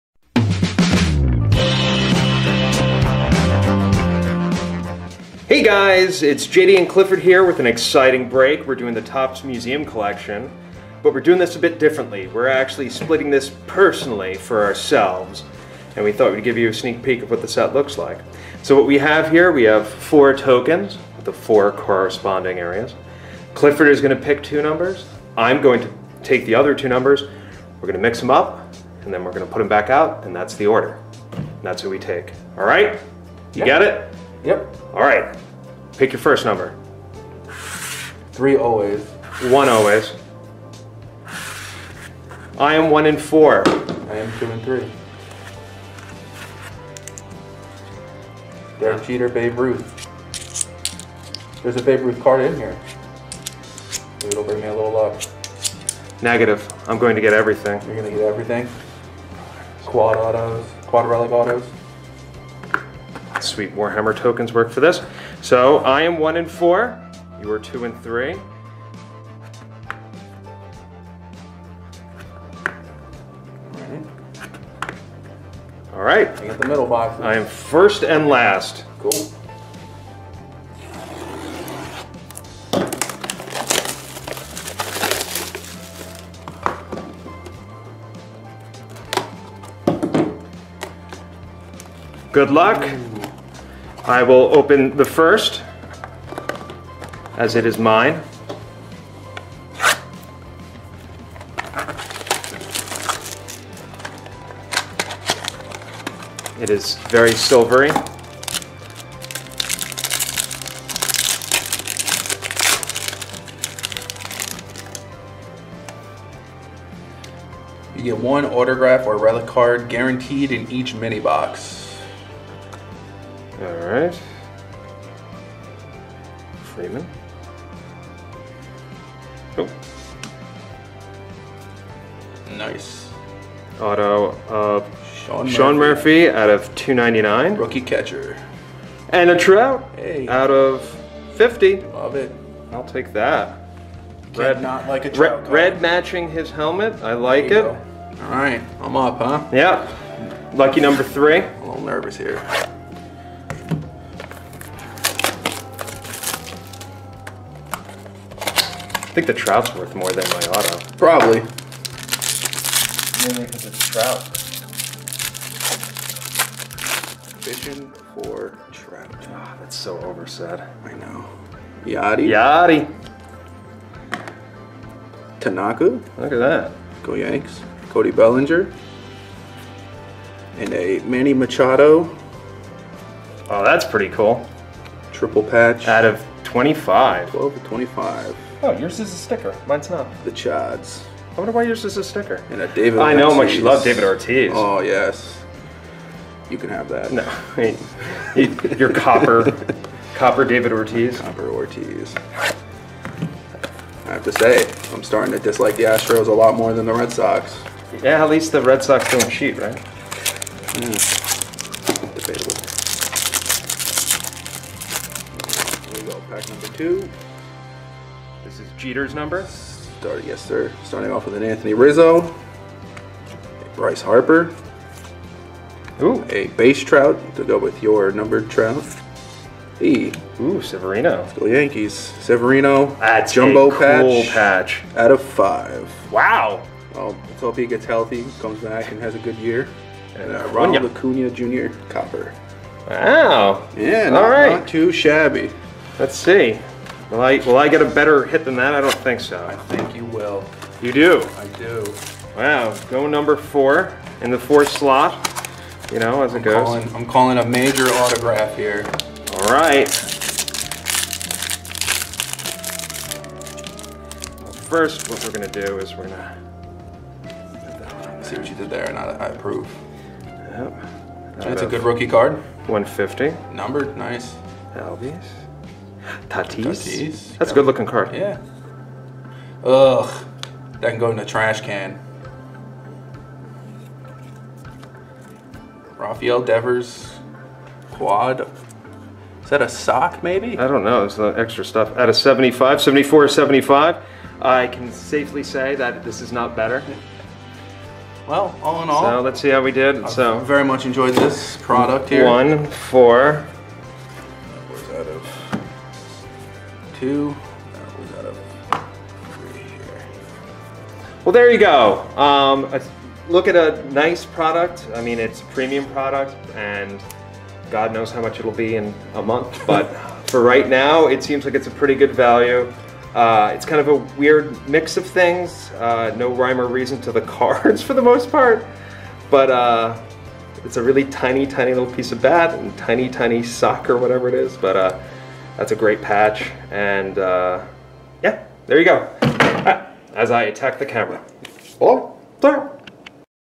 2020 Museum Collection Box Break!!
JD and Clifford break a box of the brand new Topps Museum Collection!! Stop in the store are grab a box for yourself on Friday 7/31/2020